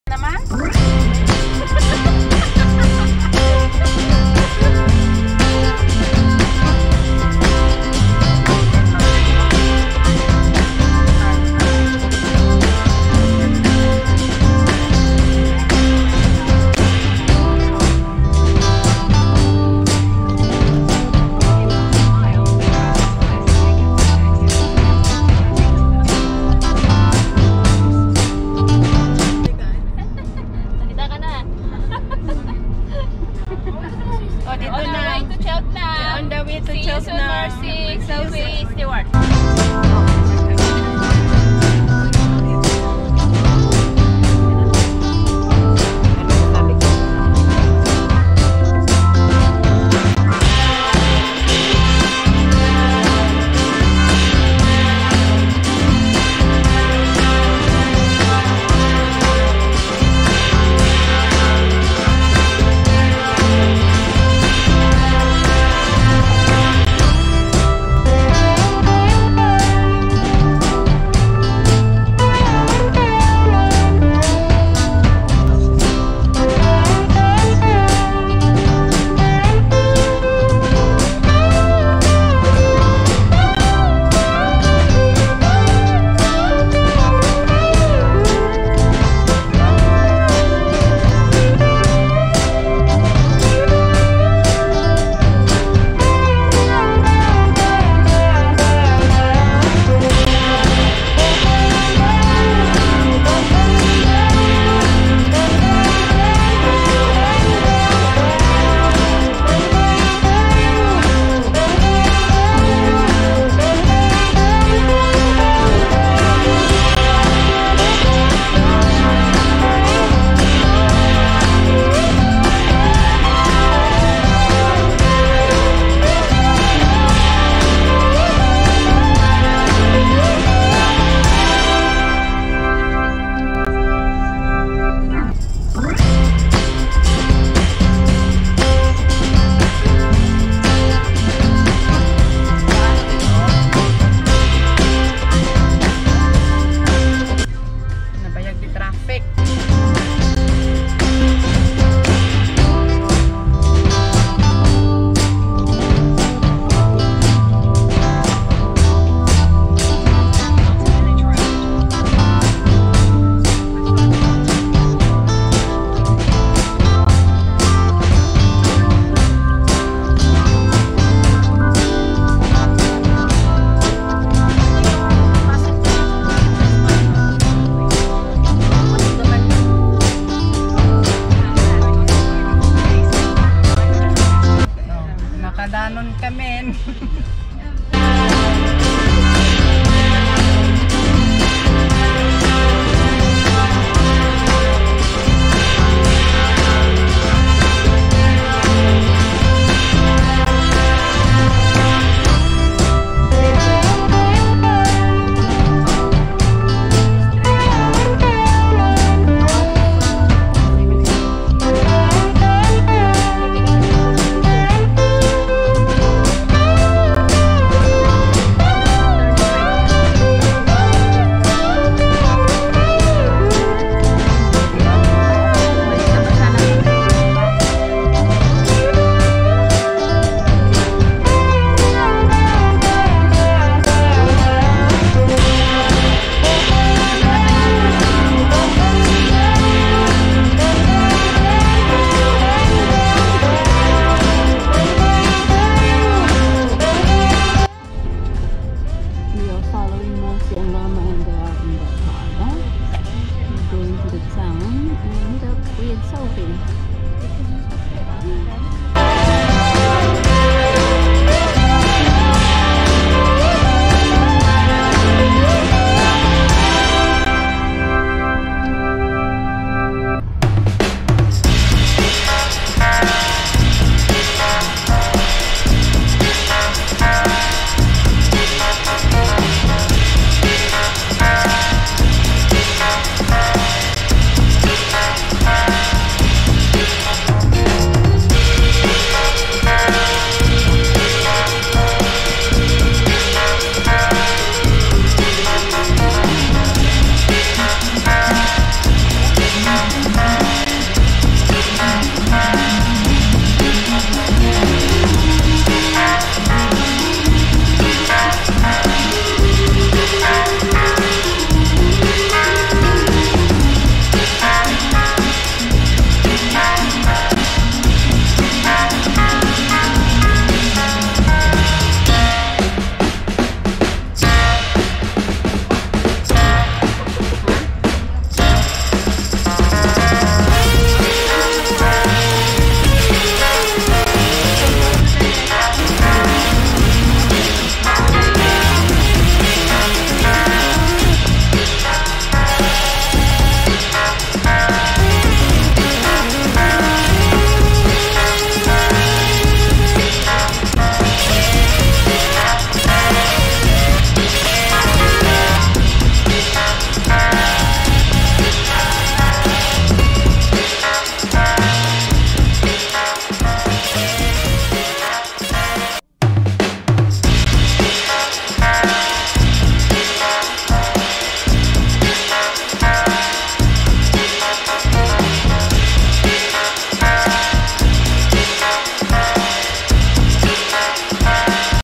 Road trip to CHELTENHAM Town, Gloucestershire, ENGLAND.
We visited a friend who used to live in HEREFORD, now she lives in CHELTENHAM. It is a town in Gloucestershire, in ENGLAND. This town is known for its annual Horse jump racing every year (month of March) held at Cheltenham Racecourse. Loads of shopping stores and good eating places, and a big public park which is good for chilling out with friends and family.